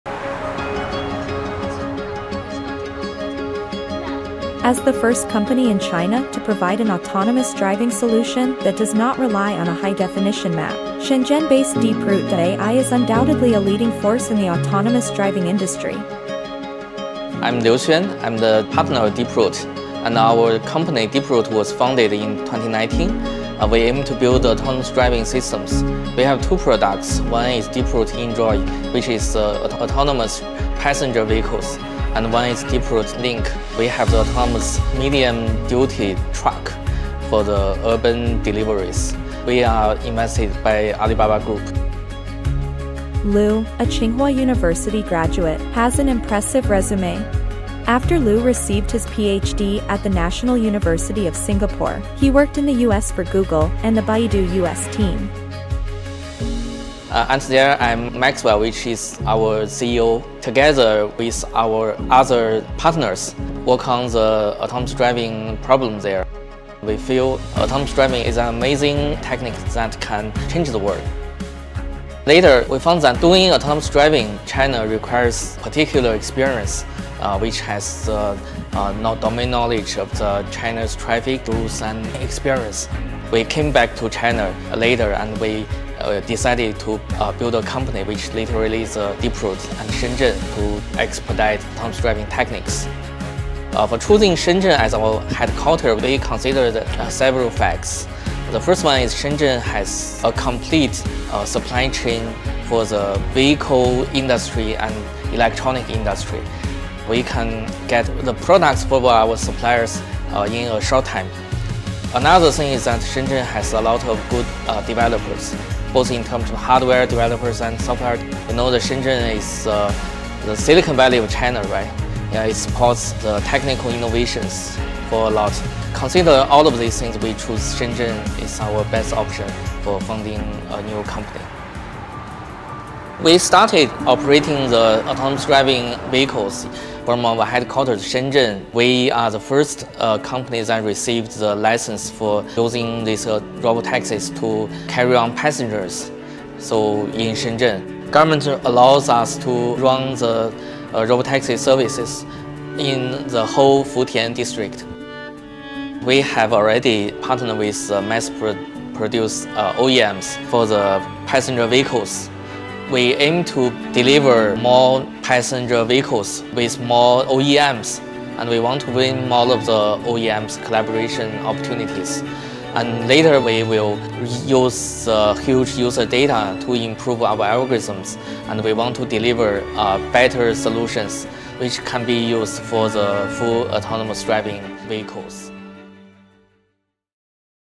DeepRoute ai in China
The first company in China to offer an autonomous driving solution that does not rely on a high-definition map, Futian-based DeepRoute.ai is undoubtedly a leading force in the autonomous driving industry. Check out this video to learn more about the company’s technology.